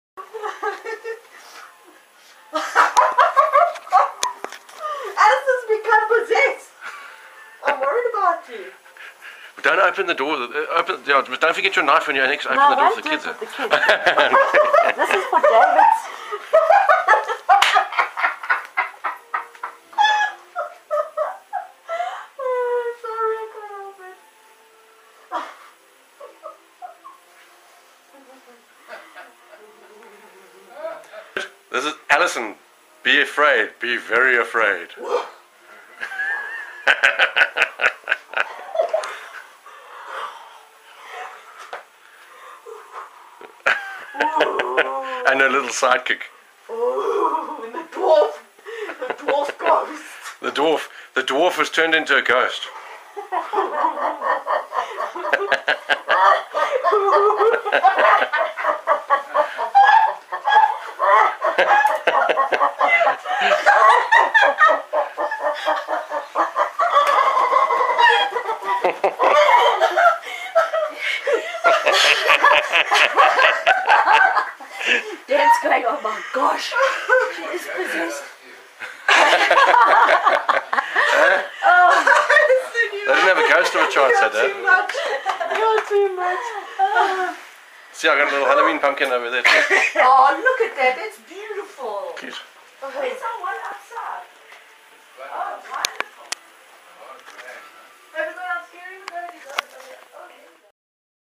Halloween UK 2010
Halloween night 2010 trick or treat, Shelley and Allison scaring the neighbourhood...